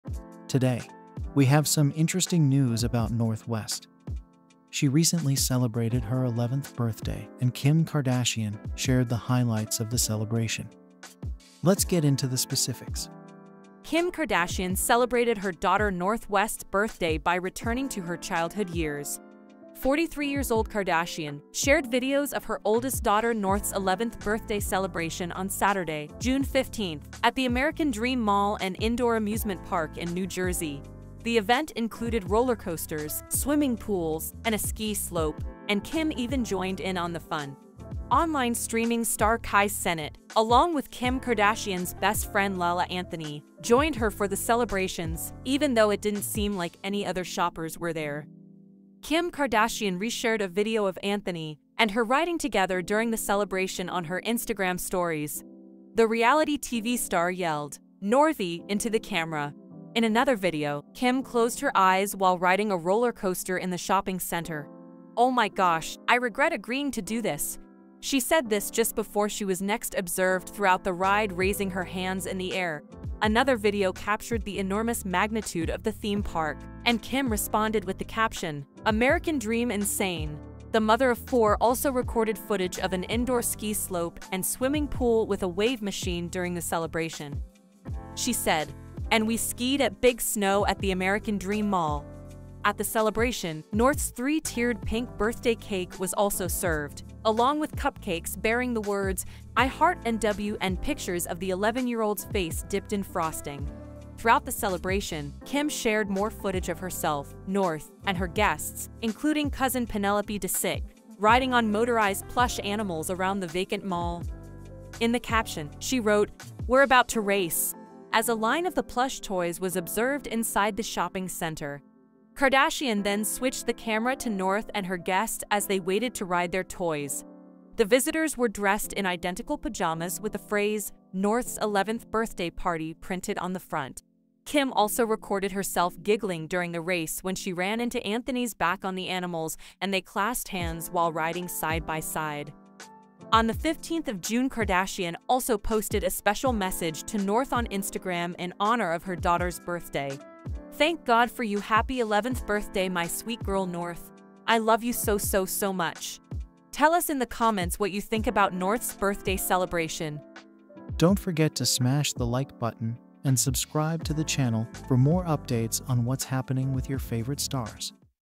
Inside North West's 11th Birthday Celebration with Kim Kardashian
Get an inside look at North West's 11th birthday celebration with her mom, Kim Kardashian. Catch up on all the latest celebrity and entertainment news right here!
Kim Kardashian celebrated her daughter North West's birthday by returning to her childhood years. 43 years old Kardashian, shared videos of her oldest daughter North's 11th birthday celebration on Saturday, June 15th, at the American Dream mall and indoor amusement park in New Jersey. The event included rollercoasters, swimming pools, and a ski slope, and Kim even joined in on the fun.